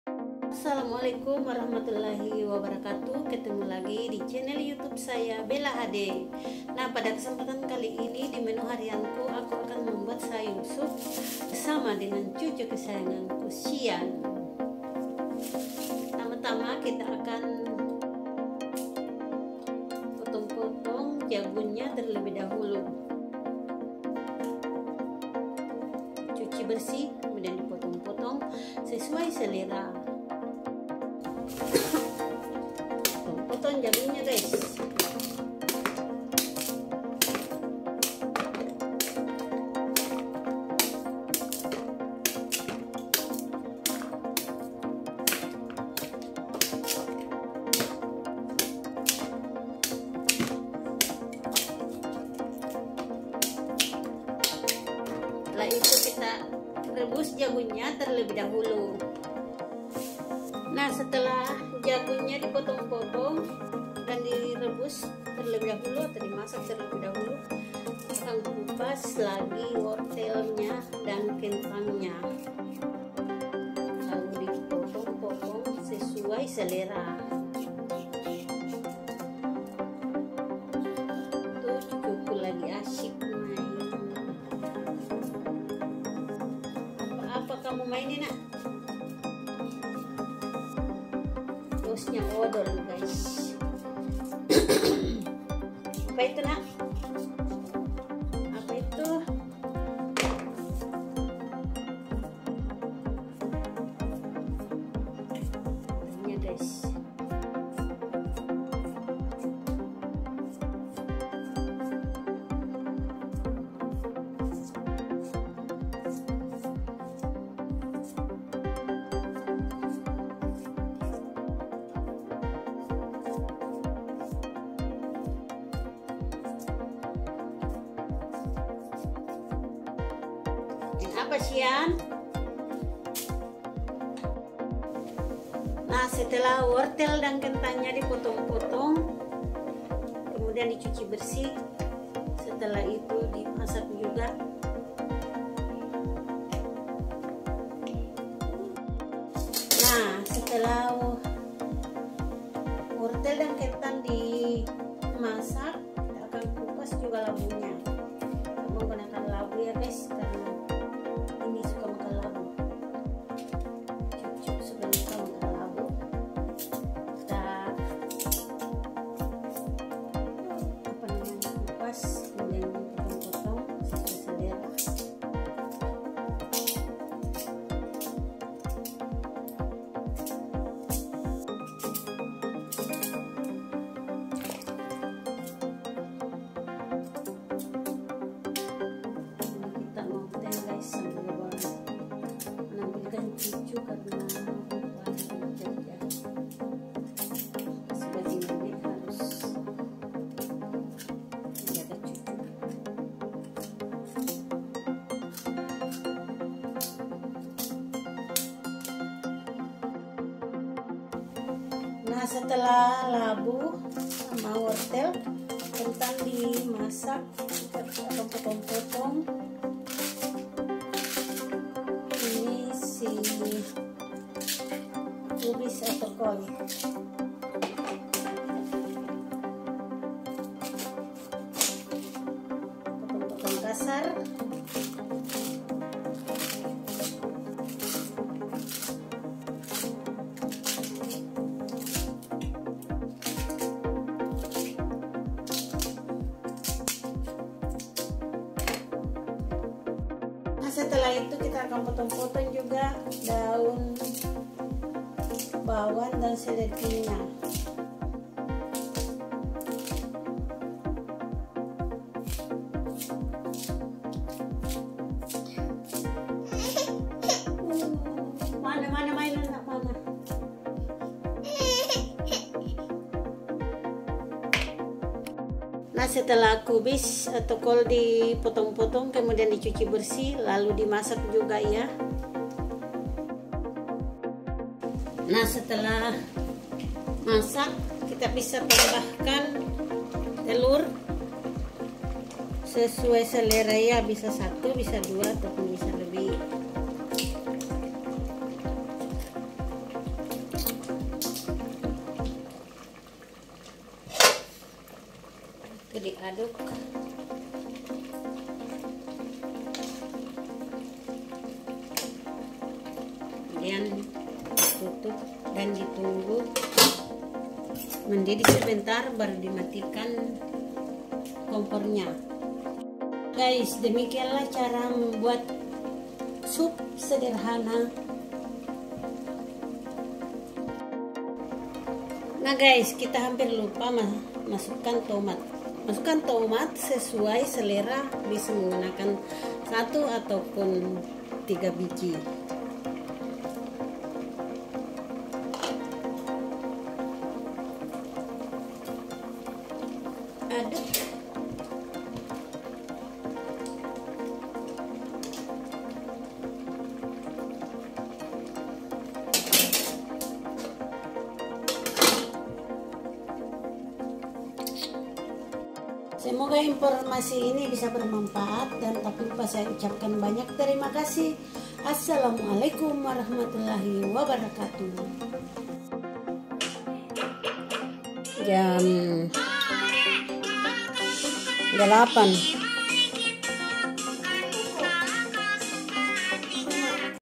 🔴 Keseharianku membuat menu hari-hari sup sangat sederhana bersama cucuku
Keseharian ku membuat menu hari-hari sup sangat sederhana bersama cucuku